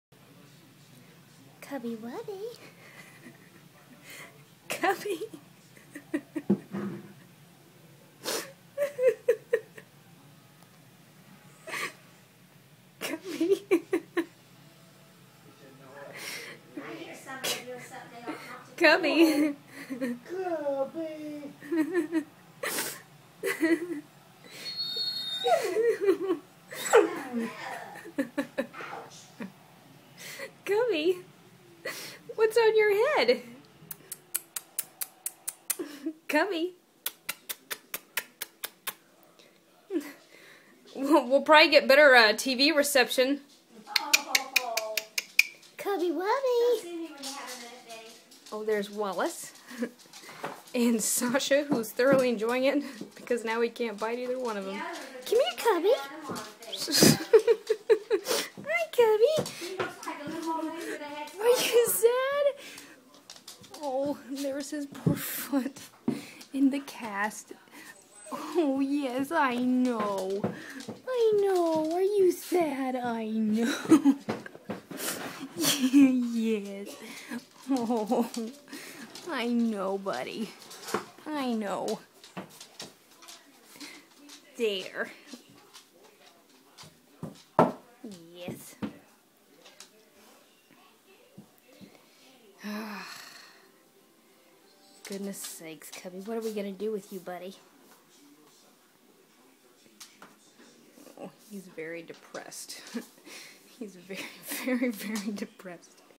Cubby and the Terrible, Horrible, No Good, Very Bad Day
Cubby somehow got a nail lodged in his back paw. He has been such a good sport throughout the entire ordeal. He passionately hates the e-collar he's got to wear for the next week though.